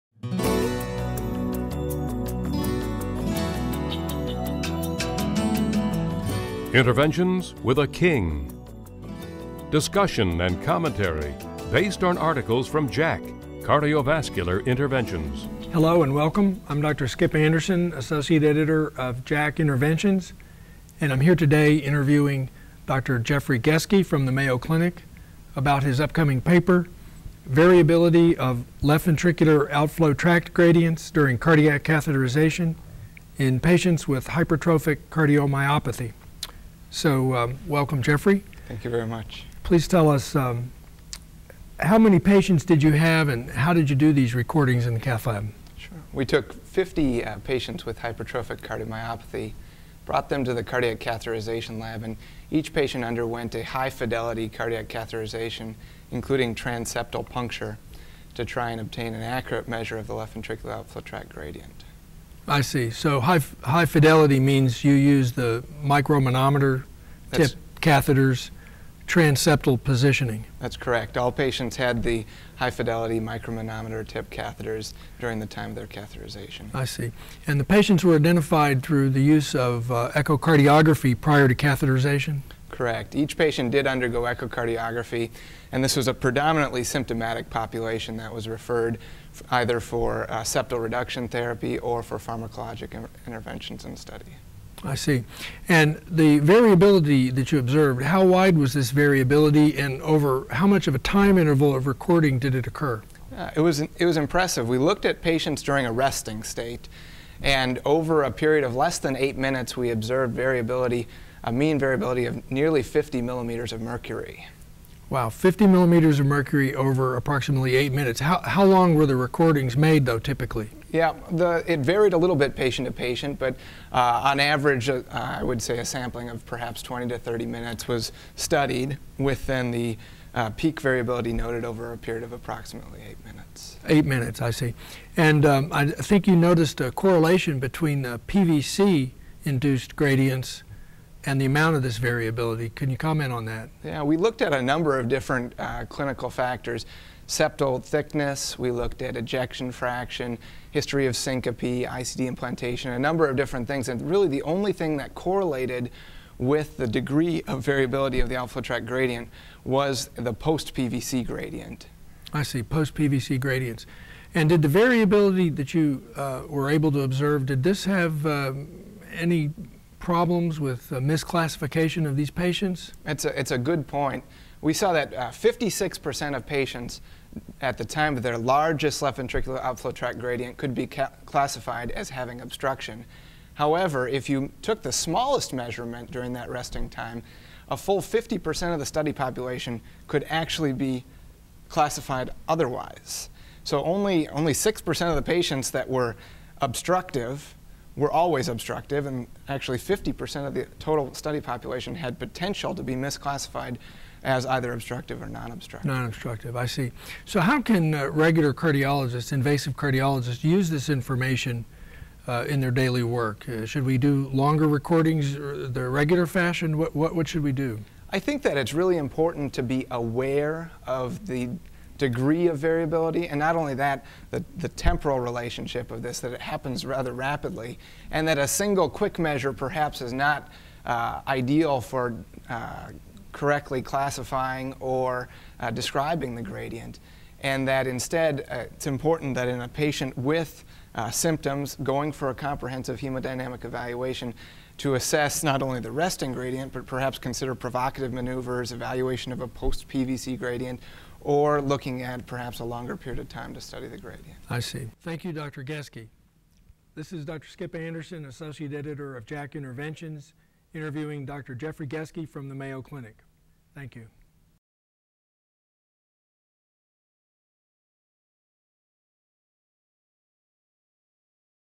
HCM LVOT Gradient Variability at Cath
Dr. H. Vernon Anderson and Dr. Jeffrey Geske discuss HCM LVOT gradient variability at catheterization. Ref: J Am Coll Cardiol Interv. 2011;4;(6).

H. Vernon Anderson, MD, FACC

Jeffrey B Geske, MD

Statements or opinions expressed in this program reflects the views of the contributors and do not reflect the official policy of the ACCF, unless otherwise noted.